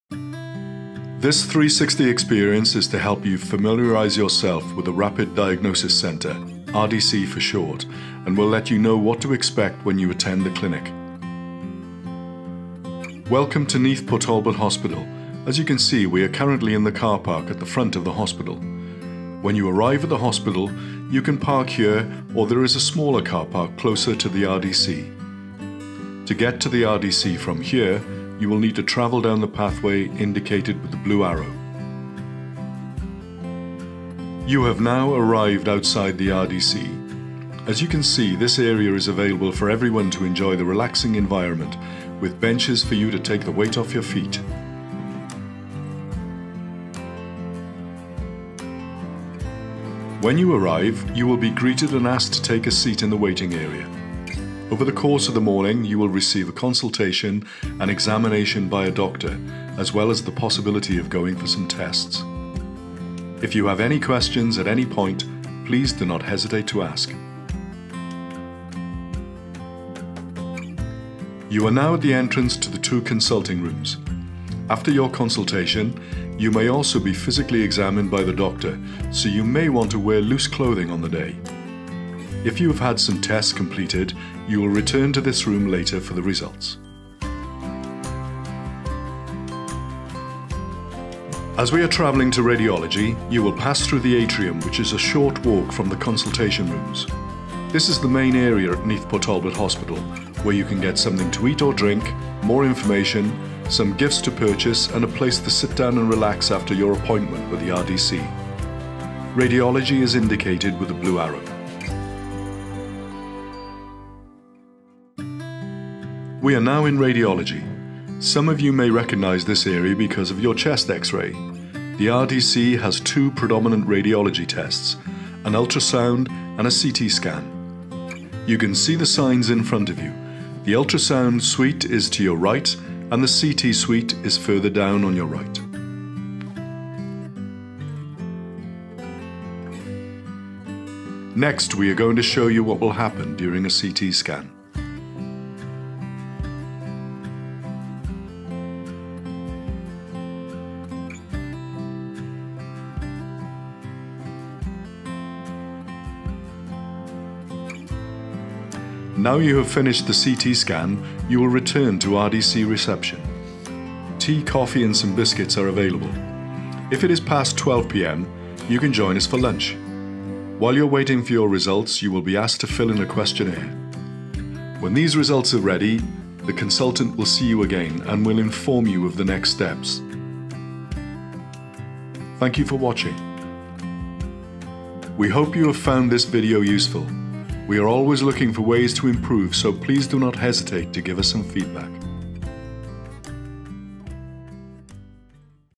A 360 degree tour of the Rapid Diagnosis Clinic at Neath Port Talbot Hospital
The step by step tour will describe  the patient journey when referred to the Rapid Diagnosis Clinic.